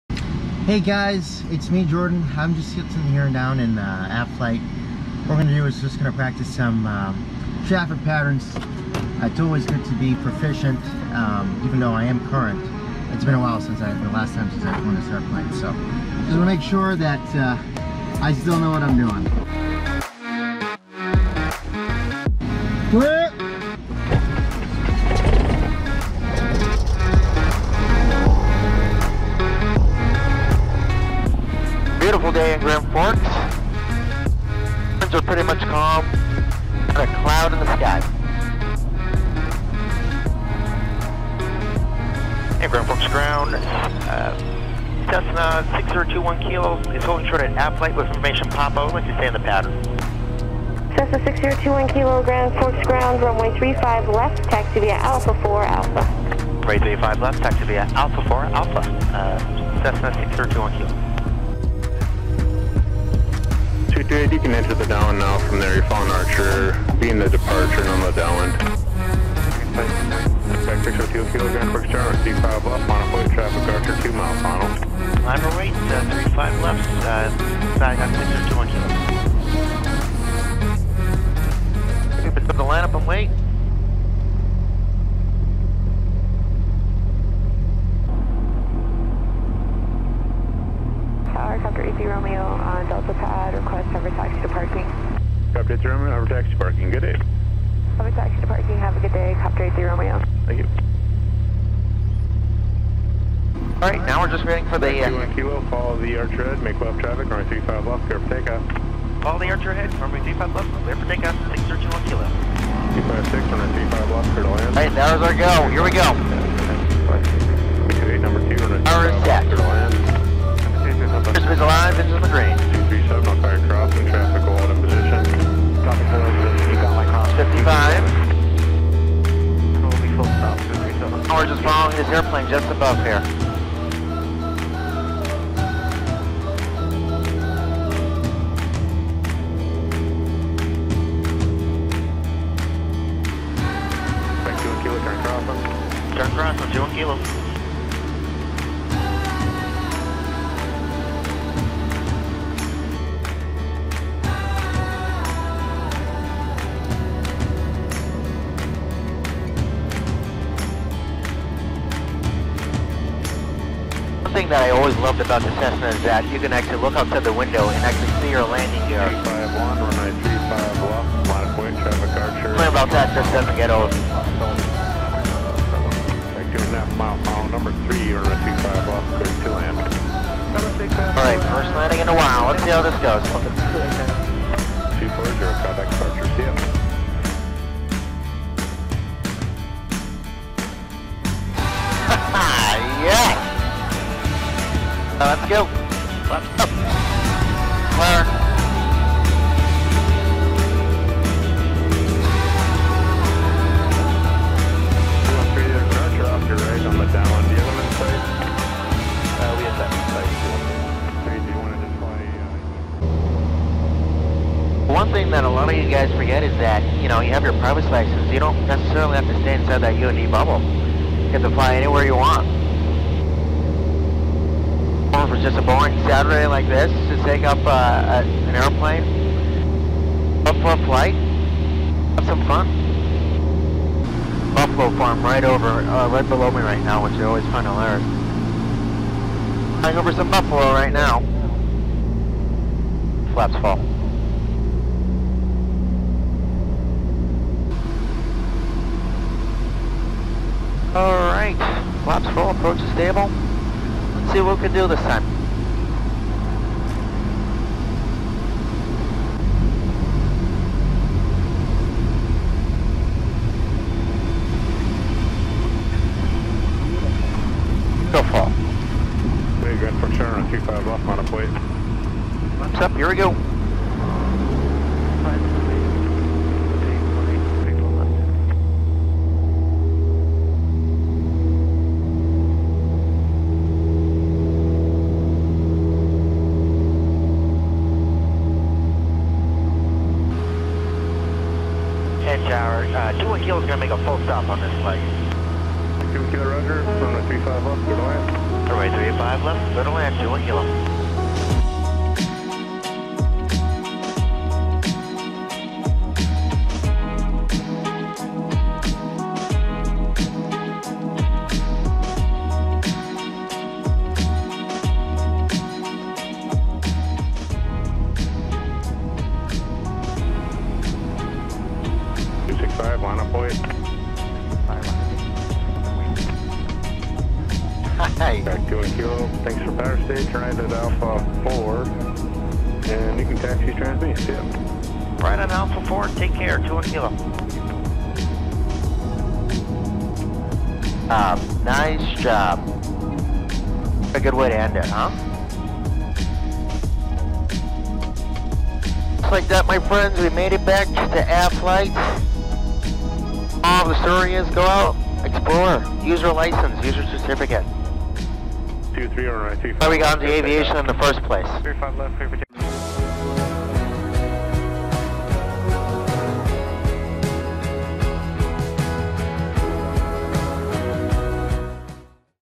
Flying the Traffic Pattern
It's always fun to just relax and go flying just for fun!!
This time I'm flying the traffic pattern!